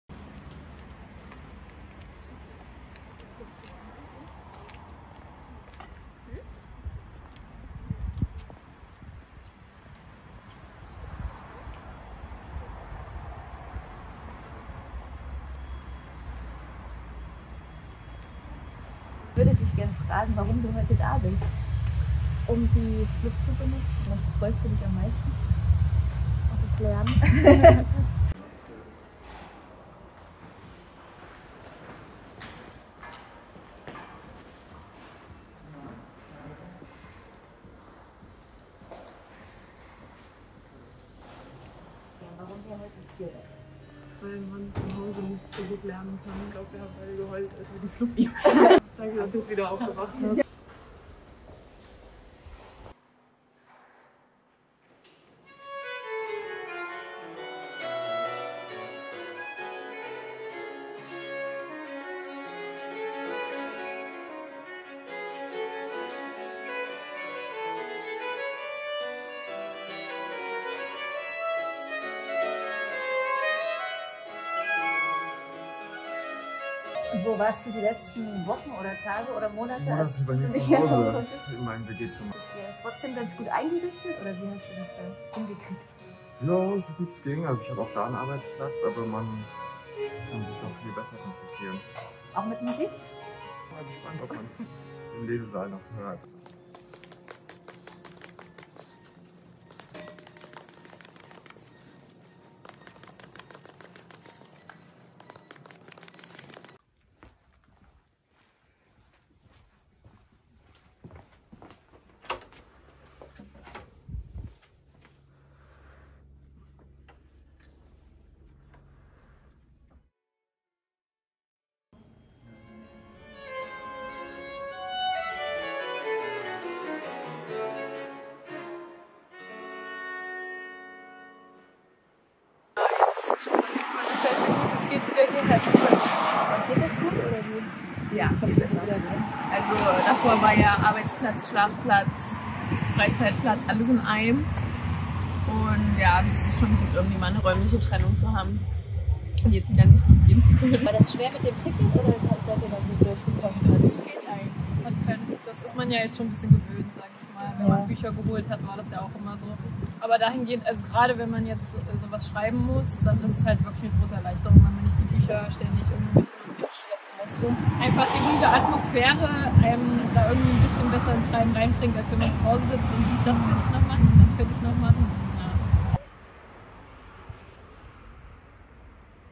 Willkommen zurück! SLUB als Lernort am 25.5.2021
Am 25. Mai 2021 konnten wir die SLUB nach längerer Corona-Pause endlich wieder als Lernort öffnen! ❤️

Zu diesem Anlass hatten wir etwas Besonderes für unsere Nutzer:innen vorbereitet: ein kleines Willkommenskonzert mit dem Dresdner Barockorchester. 🎻Sebastian Knebel und Margret Baumgartl spielten im Foyer der Zentralbibliothek Werke aus unserem Schrank II.

Außerdem haben wir die Chance direkt genutzt, um einige Nutzer:innen zu befragen, wo und wie sie in den vergangenen Monaten gelernt haben und was es für sie bedeutet, endlich wieder vor Ort arbeiten zu können.

Die wichtigsten Eindrücke vom Tag gibt es in diesem Film!